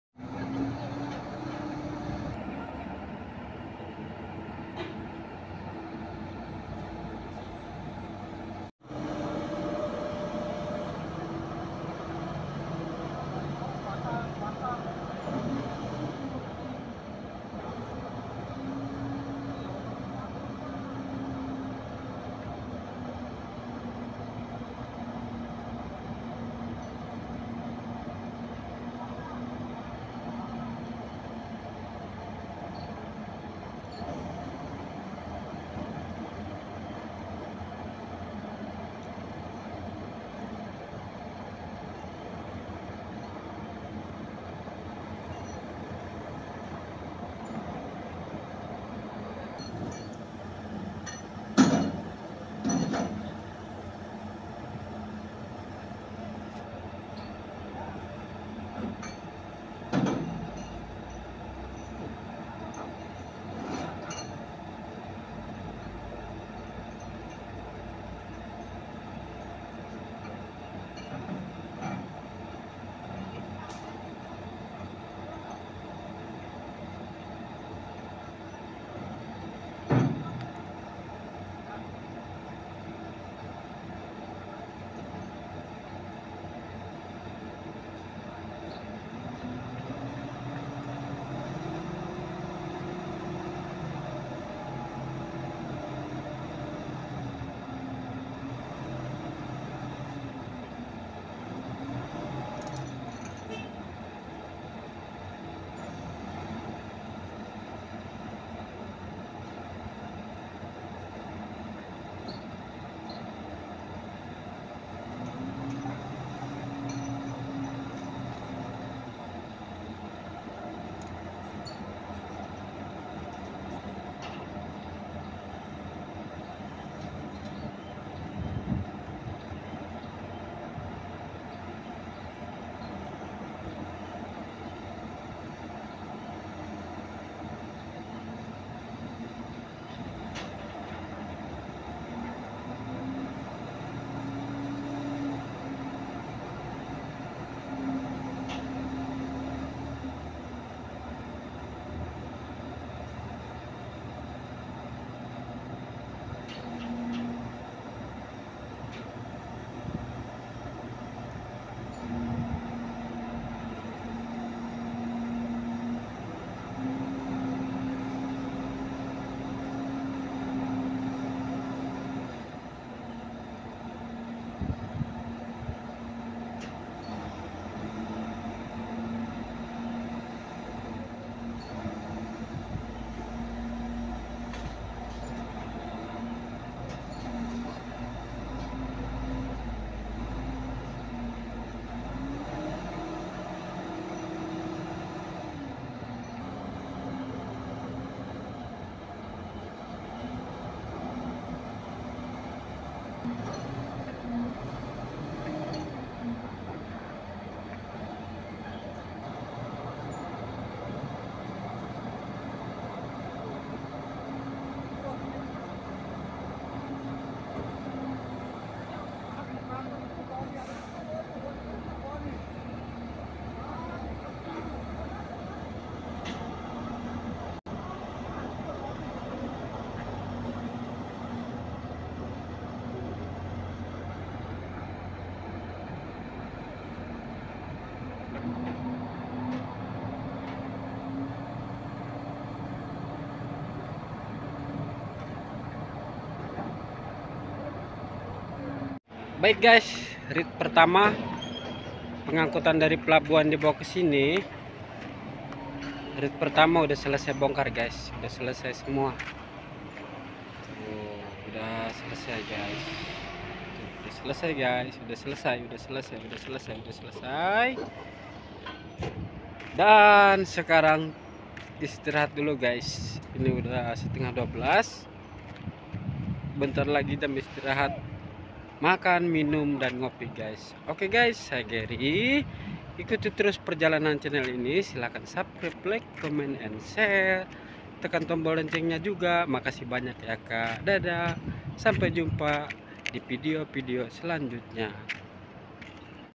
Crane Kato - Sedang Bongkar Muatan Truk Trailer - BONGKAR MUATAN TRUK
Crane Kato Sedang Bongkar Muatan Truk Trailer - Barang2 Ini Untuk Di Bawa Ke PLTU Baturaja Sumatera Selatan - Sekian dan terimakasih 🙏🙏